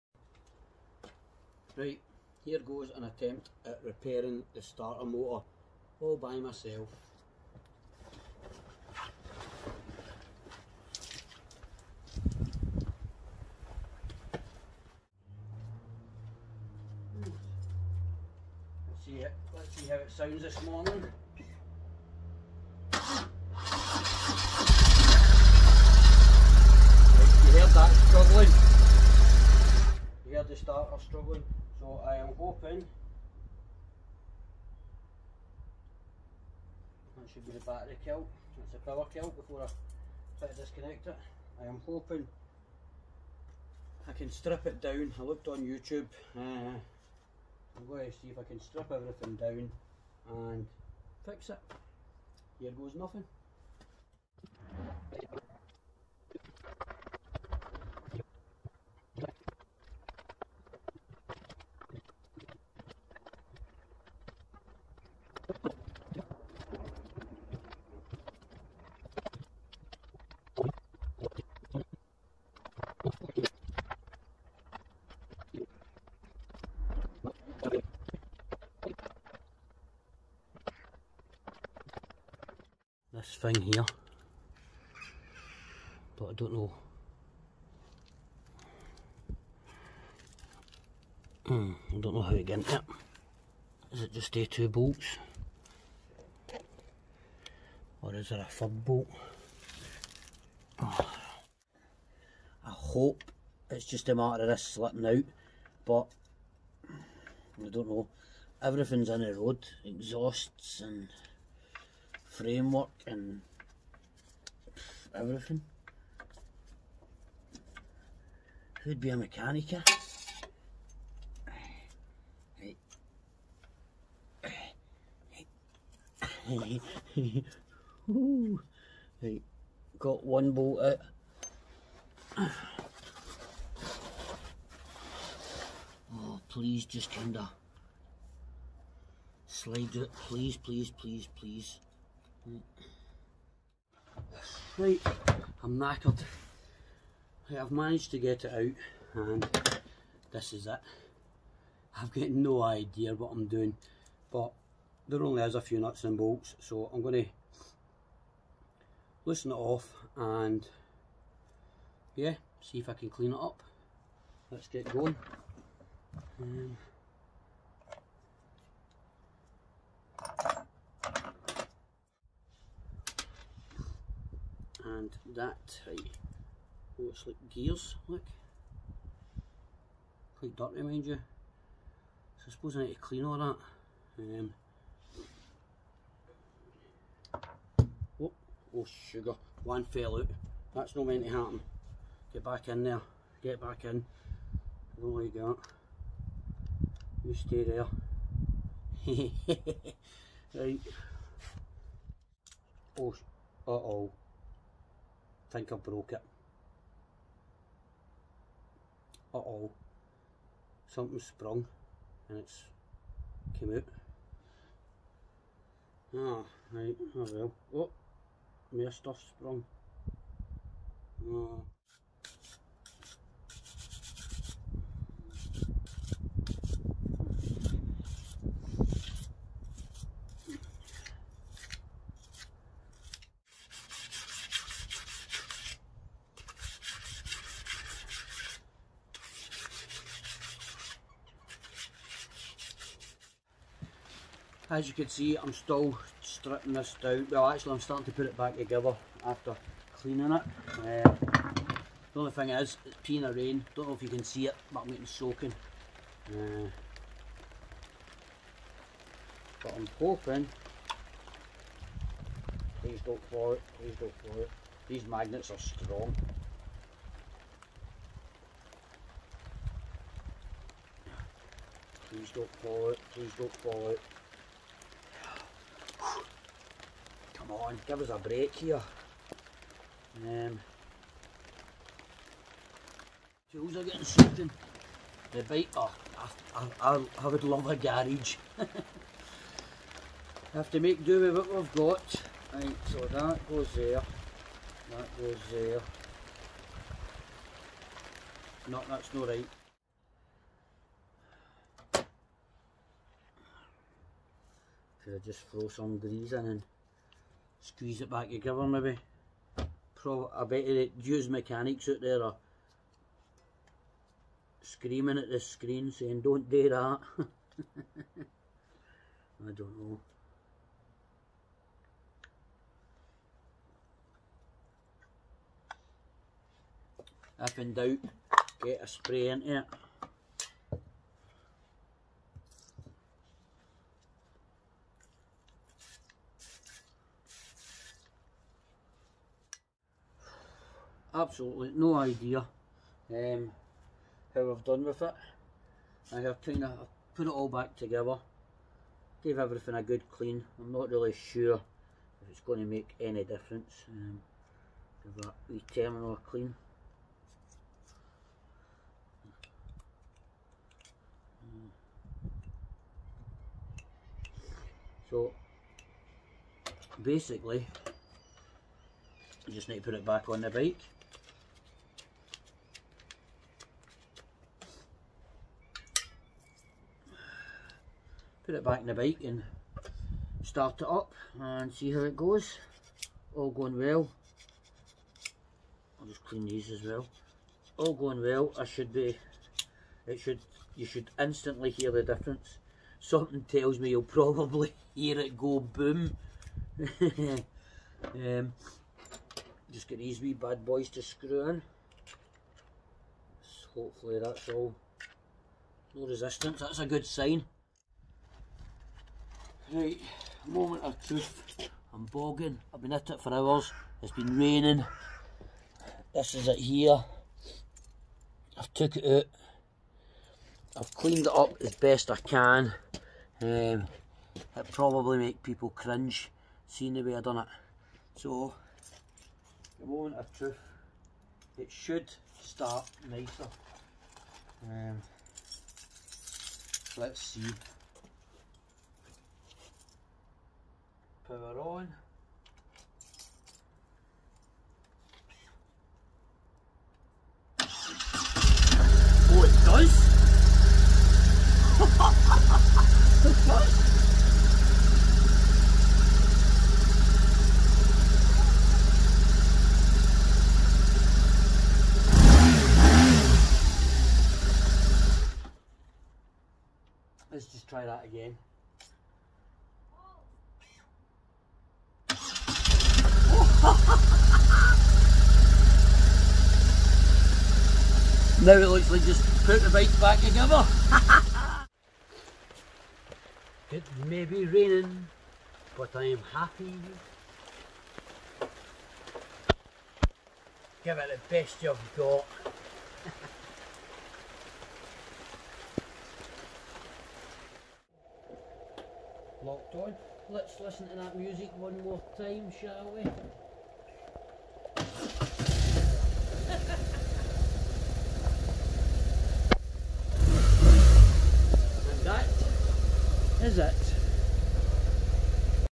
bmw r1200rt starter motor repair
starter motor keeps seizing up so trying stripping it and giving it a clean.